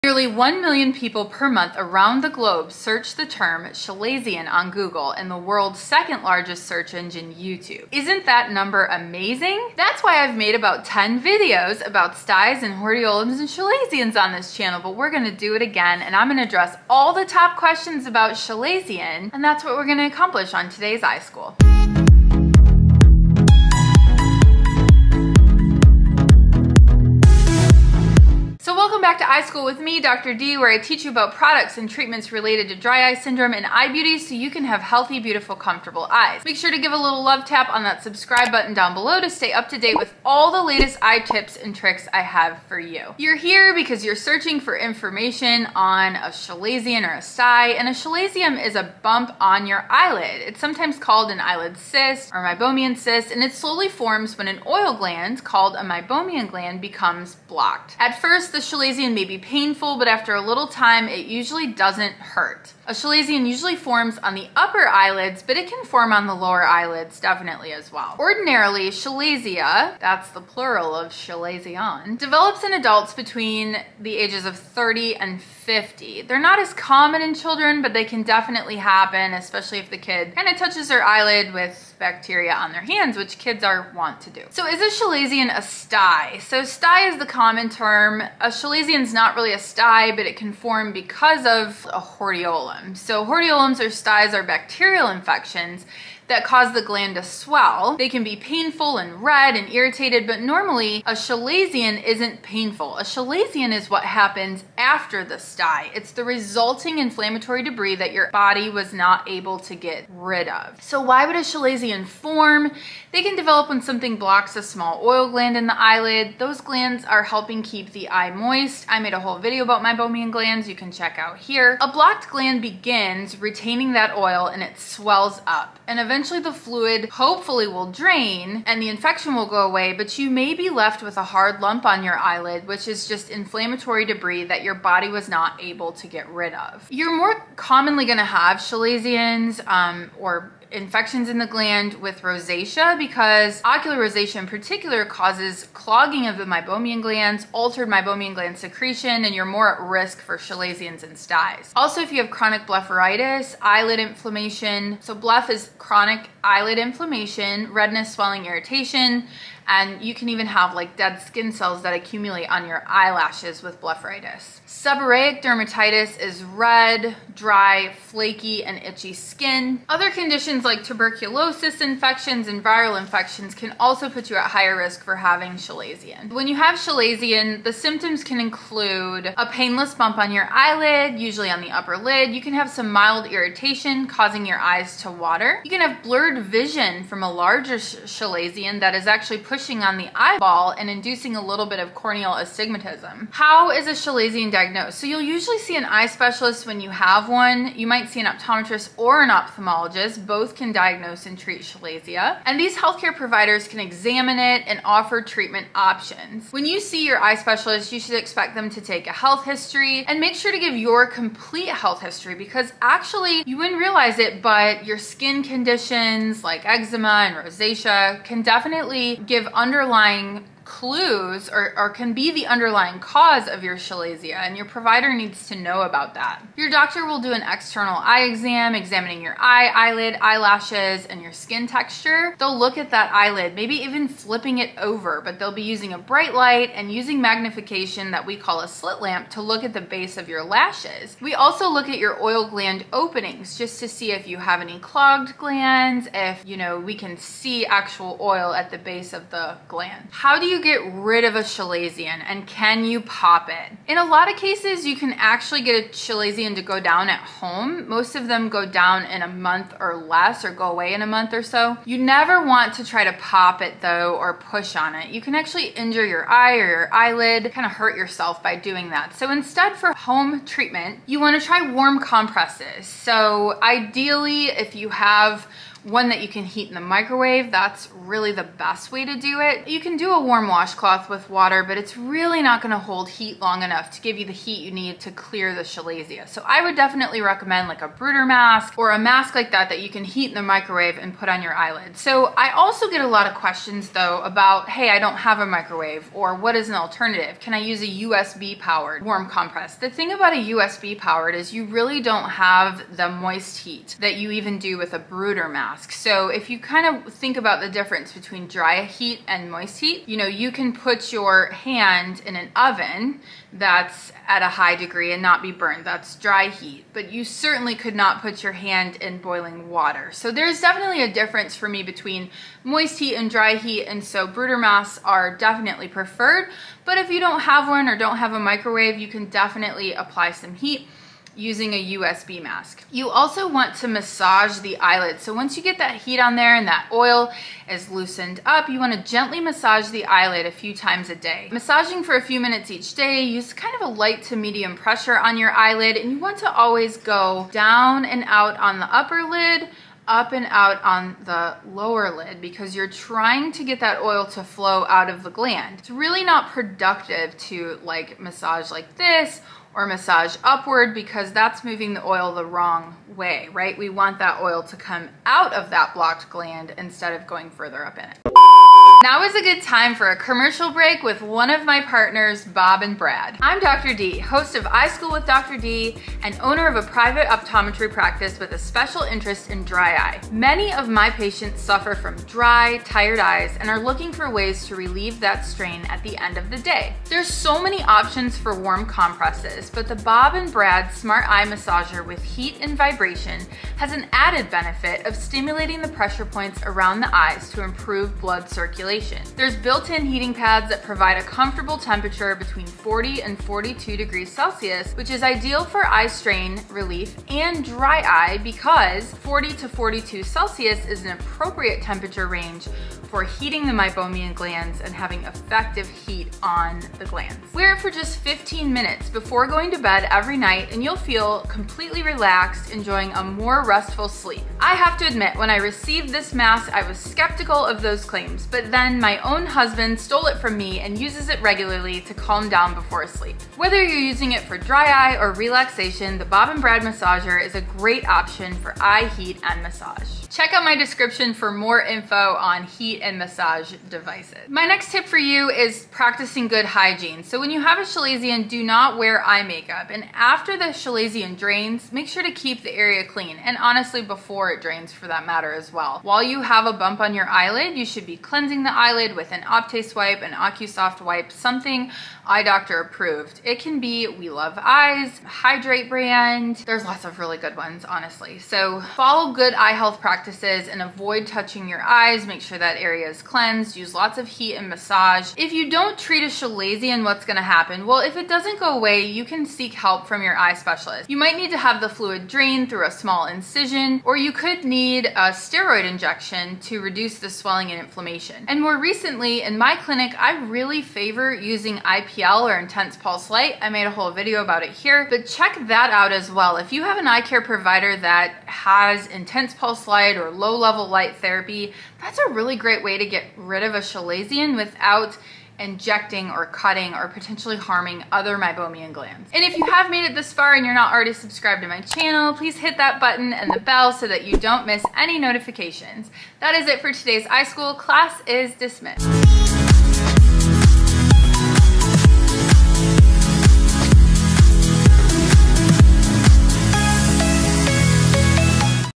How to get rid of a chalazion? What happens if a chalazion is untreated? Can you pop a chalazion?
Nearly one million people per month around the globe search the term, chalazion on Google and the world’s second largest search engine, YouTube! Isn’t that number amazing?! So I address all the top questions about chalazion in this video.

Bob and Brad Eye Massager

⏱️ Timecodes:
   00:00 Intro
   00:44 What is a chalazion?
   01:24 Is a chalazion a stye?
   01:51 What are chalazion causes and symptoms?
   03:14 How is a chalazion diagnosed?
   04:10 How do you get rid of a chalazion?
   06:10 Bob and Brad
   07:24 How to get rid of a chalazion continued . . .
   07:56 What happens if a chalazion is left untreated?
   08:29 Summary

A chalazion is a red bump on your eyelid. It is sometimes called an eyelid cyst or a meibomian cyst. It slowly forms when an oil gland (called a meibomian) becomes blocked. At first, the chalazion may be painful, but after a little time, it usually doesn't hurt. Chalazia may develop when something blocks a small oil gland in the eyelid. These glands help keep the eye moist. A blocked gland begins retaining oil and swells. Eventually, the fluid will drain, and you may have a hard lump on your eyelid.